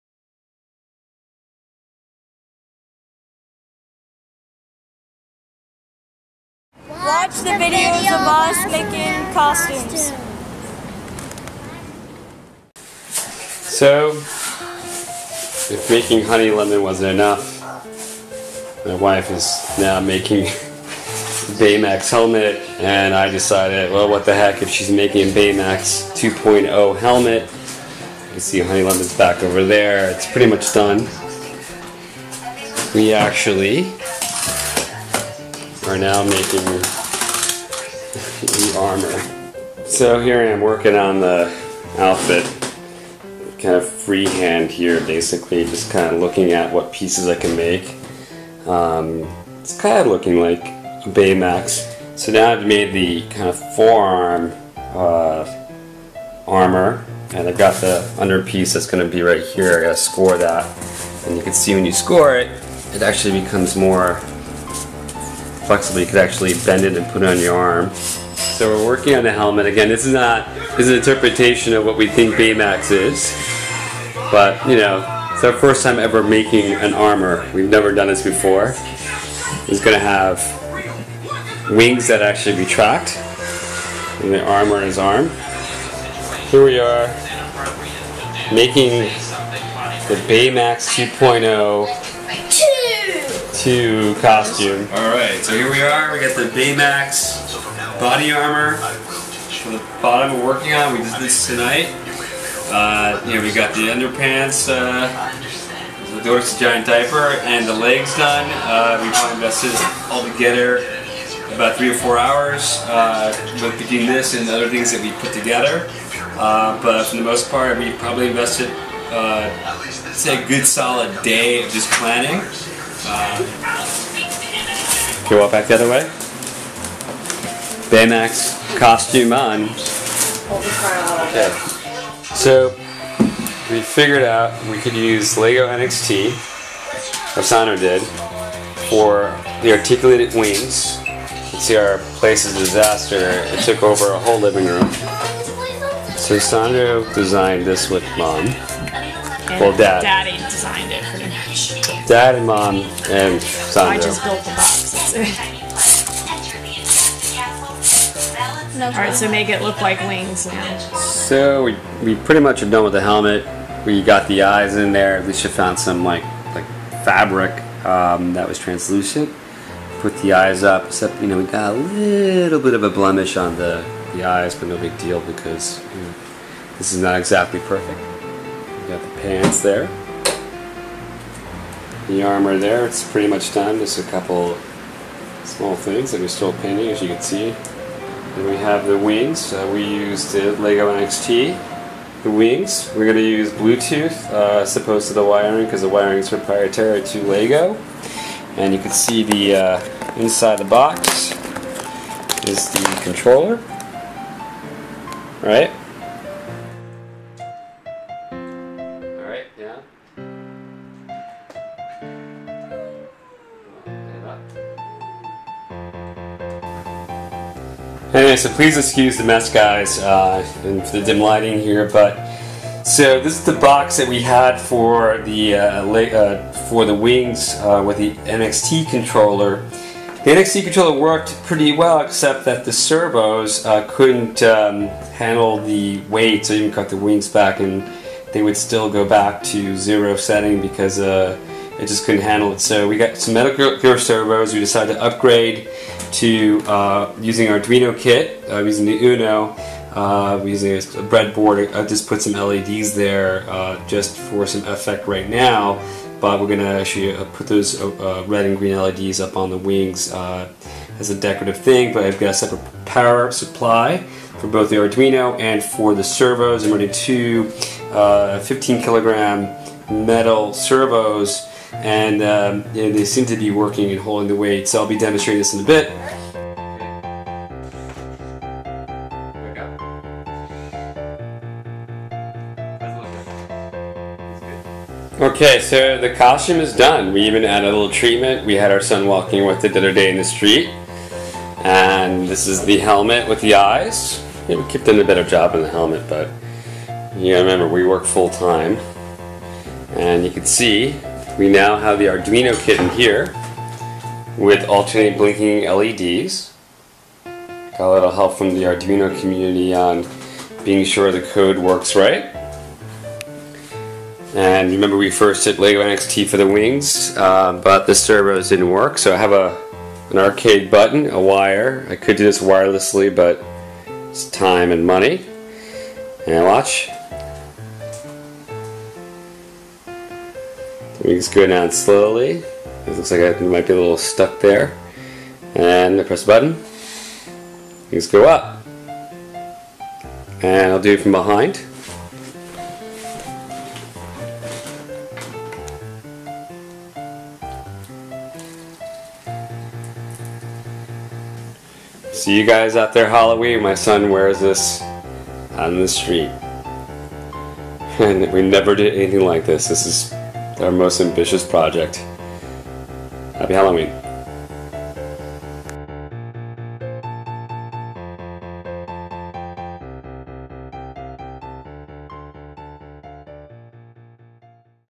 Big Hero 6 - How to make Baymax 2.0 Costume with Honey Lemon
Our kids convinced us that they wanted to be Baymax and Honey Lemon for this Halloween. After looking at a number of costume stores, we noticed there was nothing of any quality that we would want to put our kids in.  We did buy the Honey Lemon outfit from Party City but, the only things we used were the dress, bag strap, tights and part of the visor face mask.

As a family, we make costumes rarely and this by far was our most complicated to make. We use LittleBits for LED's, we spent a fortune @Michaels department store on foam, clay paint etc. And through trial and error we made the Chemball Purse that lights up and dispenses balls just like Honey Lemons purse. We even made the helmet with cardboard, crayola modeling clay, Blick paint, Modge Podge and our sweat...

For Baymax 2.0, we first tried to use the LEGO NXT controller we had to move the wings but, after assembly, we had realized the servos were not strong enough to lift and keep the wings in place. After hours of debating a strategy, we all decided it was time to brush up our skills in Arduino and get some good servos to move the wings and light LEDs - good thing dad knows how to code some.

With loads of sweat and time - we finally got the wings working right with a little help from the Arduino community.

We hope you like how we made this costume and inspired you to make your own. We are looking forward to getting better at making costumes.

This was one our most favorite family projects to date!